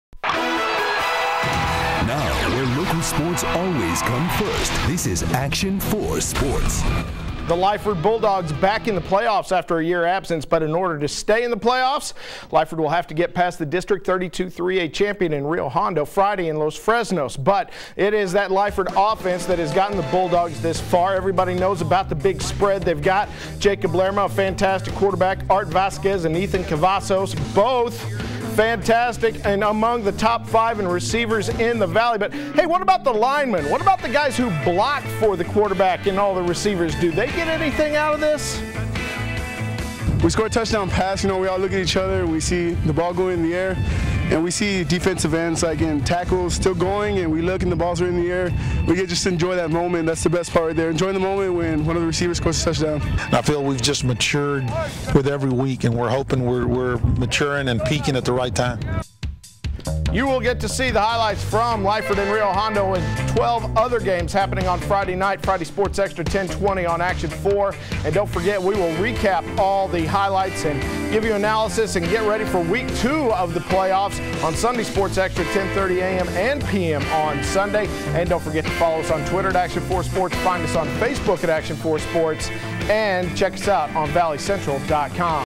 Lyford Linemen Enjoy Bulldogs Spread Offense
While the Lyford quarterback and wide receivers get the statistics benefit from running coach Jaime Infante's spread offense, the Bulldogs linemen share in the glory in their own way.